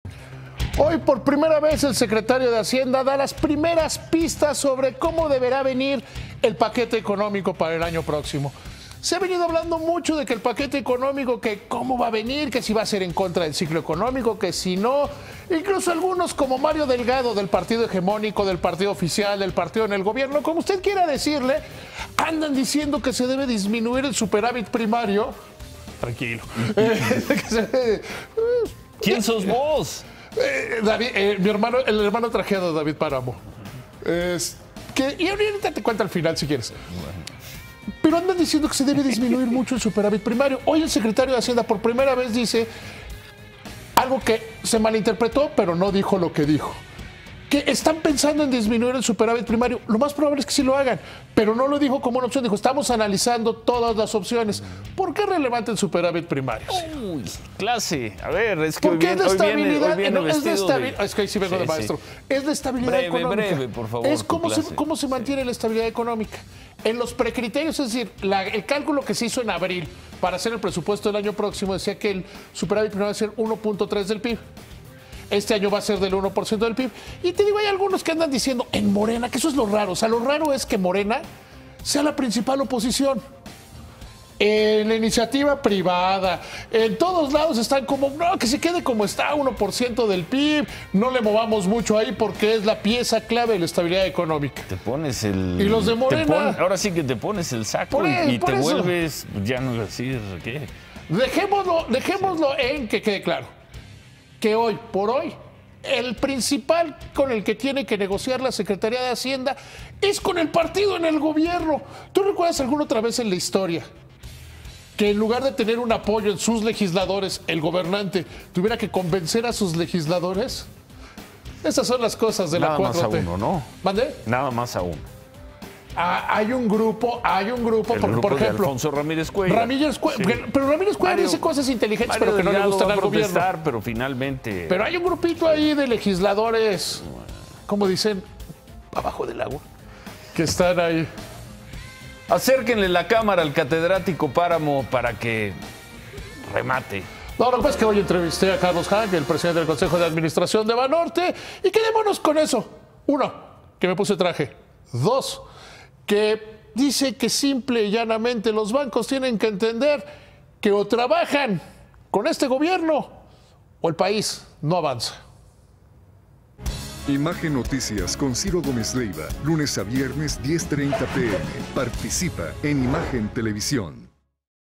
¿Cuáles son las pistas del paquete económico de México en 2020? | Ciro Gómez Leyva
¿Primeras pistas para el paquete económico del próximo año? David Páramo tiene la información: